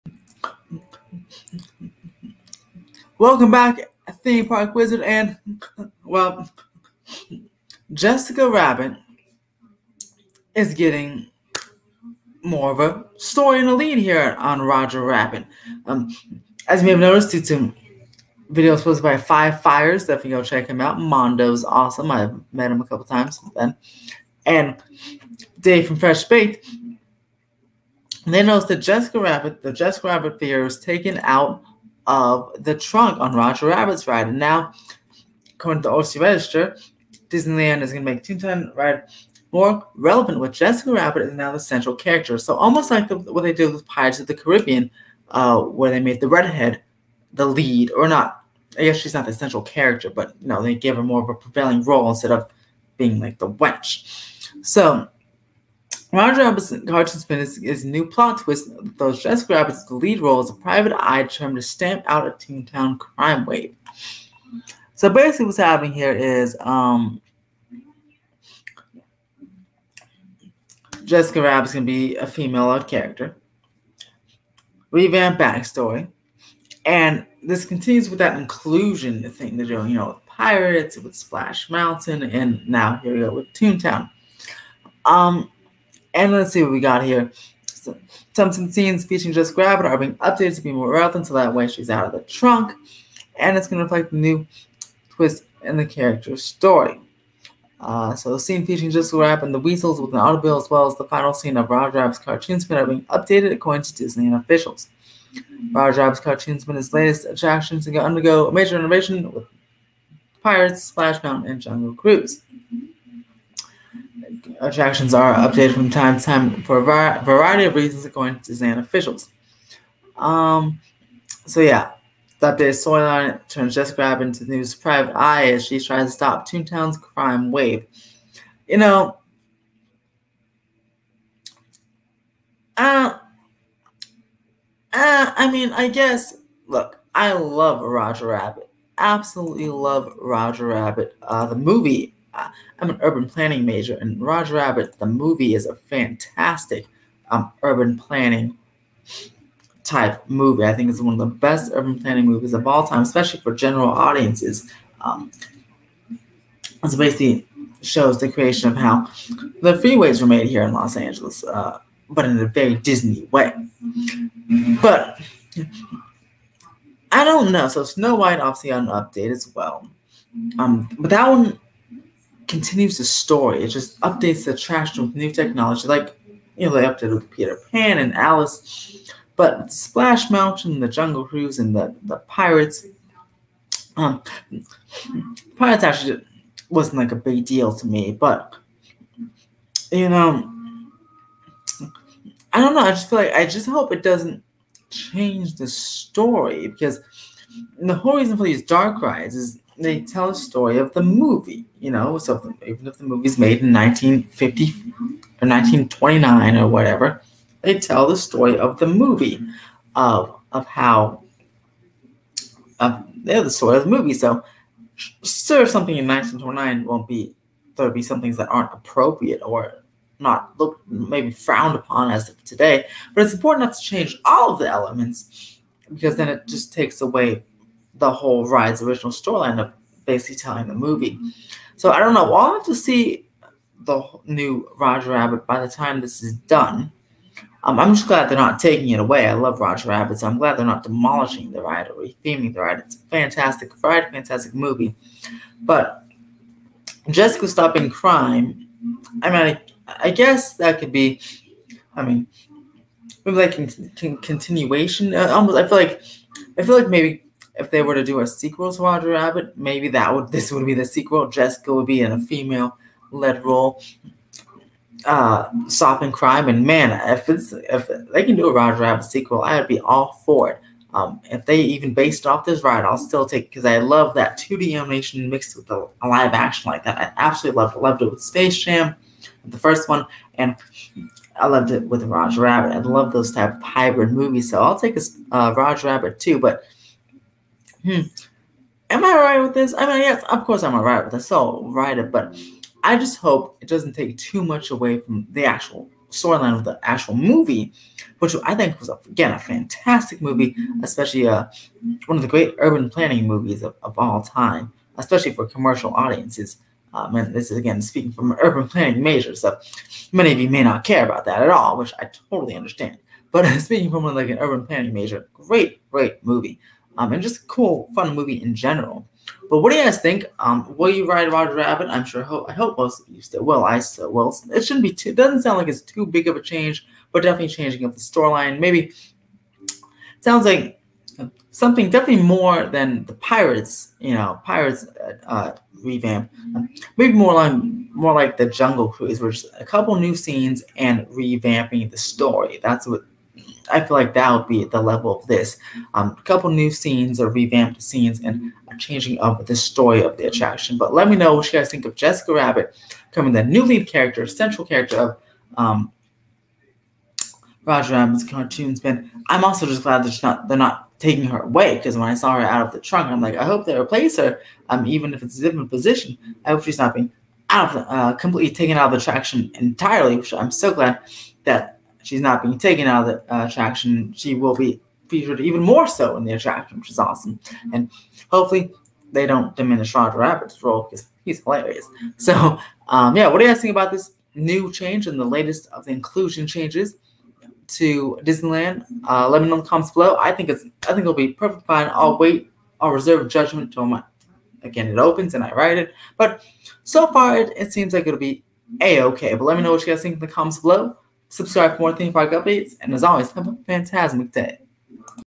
Roger Rabbit Getting Jessica Lead Refurb! | Disneyland Park | Toontown
disney is changing rogerrabbit cartoon spin in disneyland to be more inclusive with Jessica Rabbit taking more of a central role! What do you guys think?